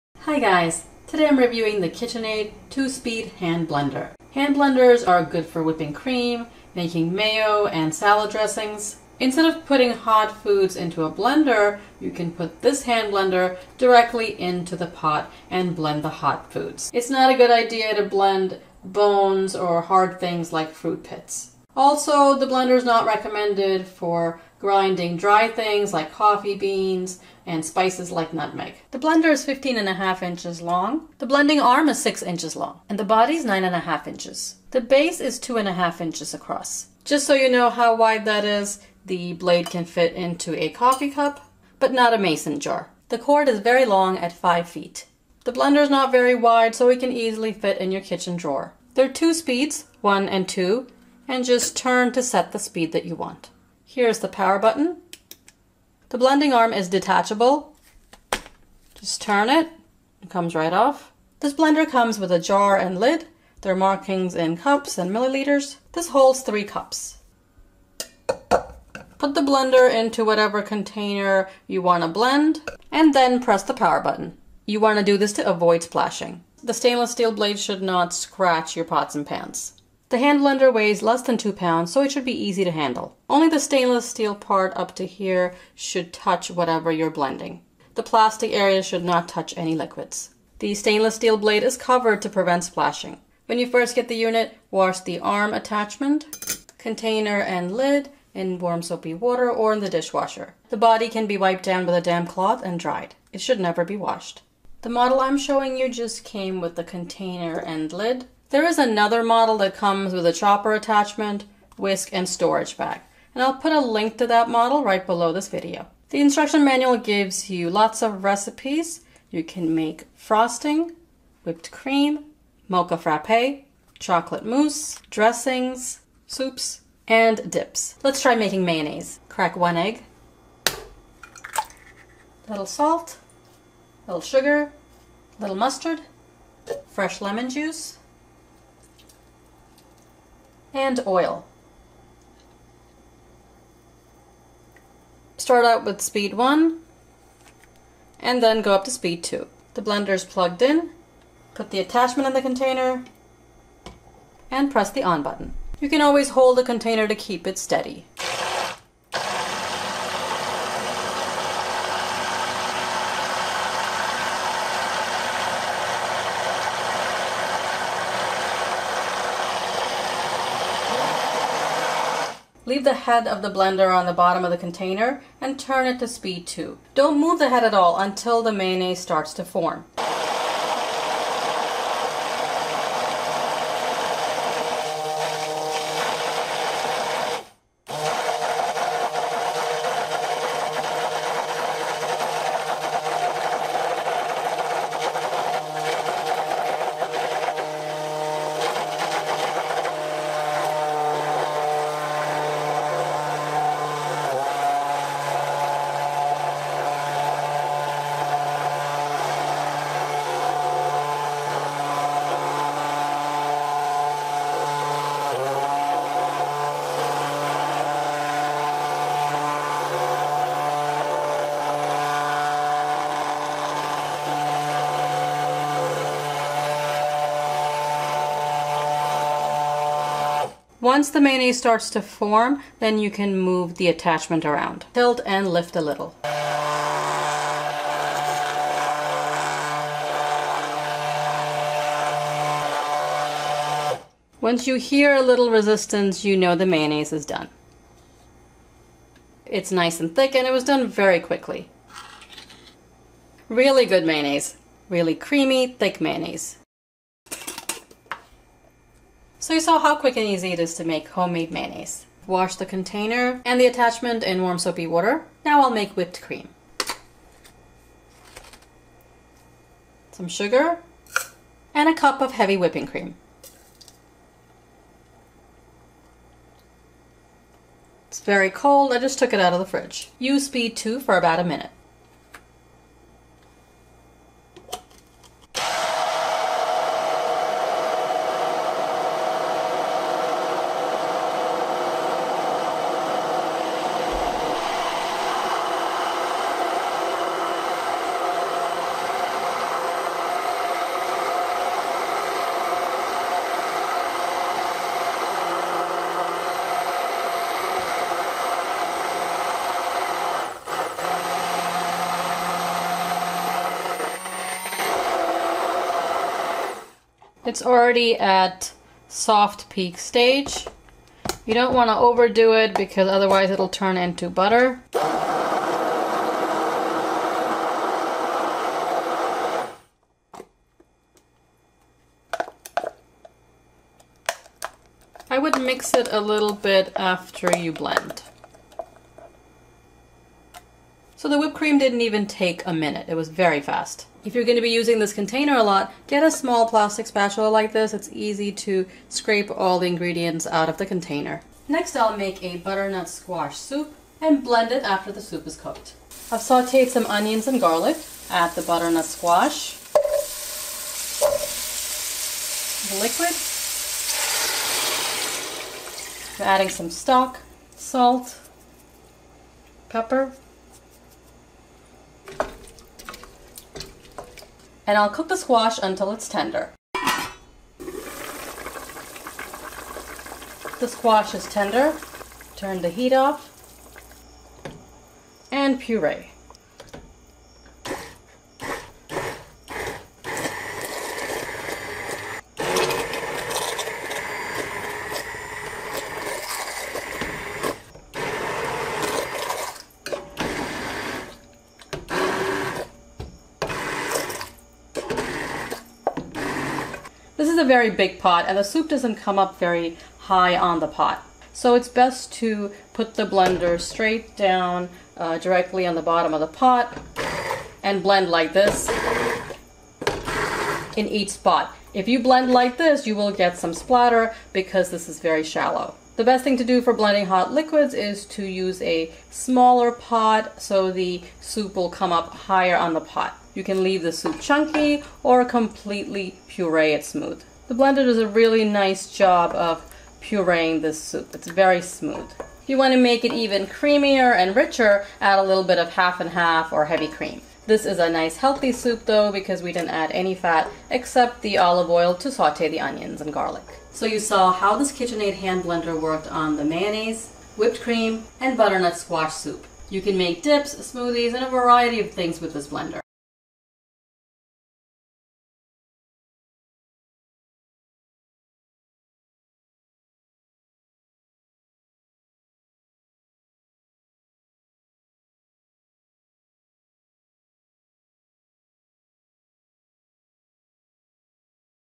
KitchenAid 2 Speed Hand Blender Review and Demo | Kitcheniest
LINK to KitchenAid Hand Blender

Hi Guys, today I’m reviewing the KitchenAid 2-Speed Hand Blender.

LINK to Blender with Whisk and Chopper
LINK KitchenAid 3-Speed Hand Blender

LINK KitchenAid 5-Speed Hand Blender

LINK KitchenAid 5-Speed Hand Blender - Brushed Stainless Steel

I’ll show you How to Make Mayonnaise, Whip Cream and Puree Hot Soup. This versatile hand blender is a useful tool in your kitchen.

Video Credit: Best Kitchen Reviews